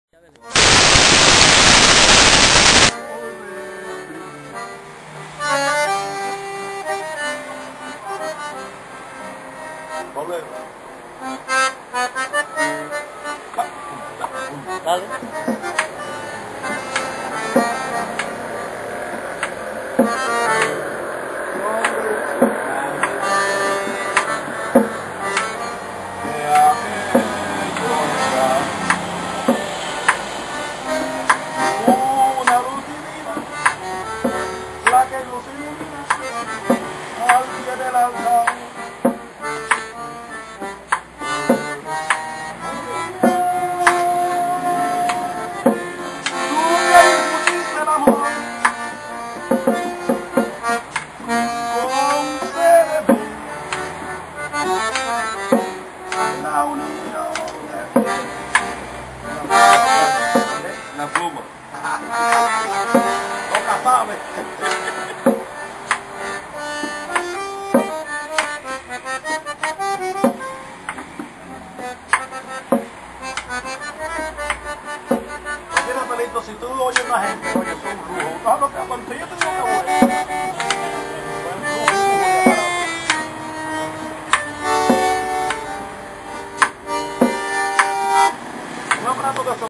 RAFAELITO ROMAN TOCA BANDOLION
RAFAELITO TOCA TODOS TIPOS DE INSTRUMENTOS, EL ESTA TOCANDO ESTA CALLE AL FINAL MIENTRA QUE EL ALUMNO ACORDEONISTA JULIO CESAR ESTEVEZ LE ACOMPAñA, CON LA TAMBORA EN SU RESIDENCIA, JUNTOS A MAS AMIGOS